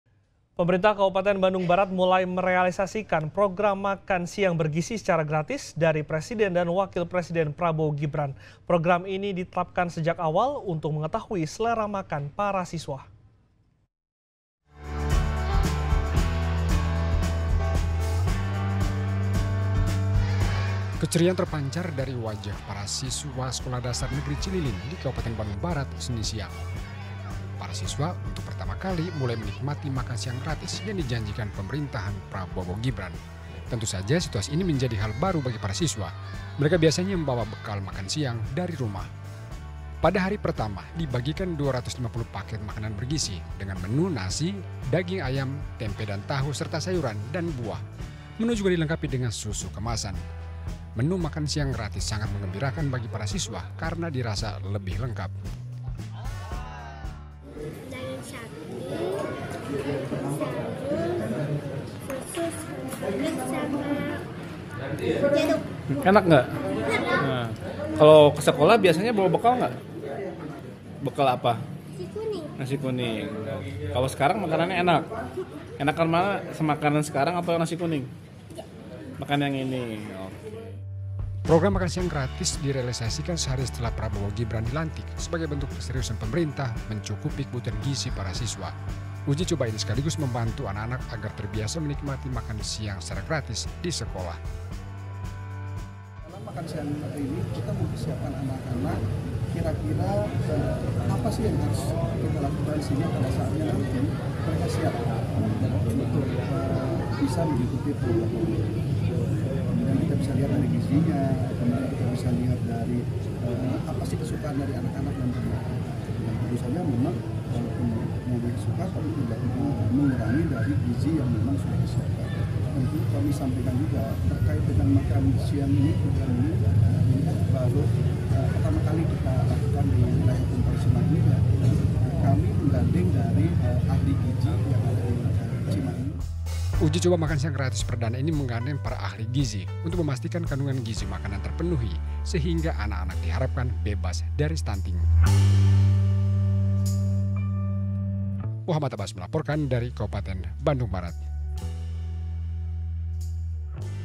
Perdana! Siswa SD Cililin di Kabupaten Bandung Barat Mendapat Makan Siang | Liputan 6 Bandung
Liputan 6 Bandung, 22 Oktober 2024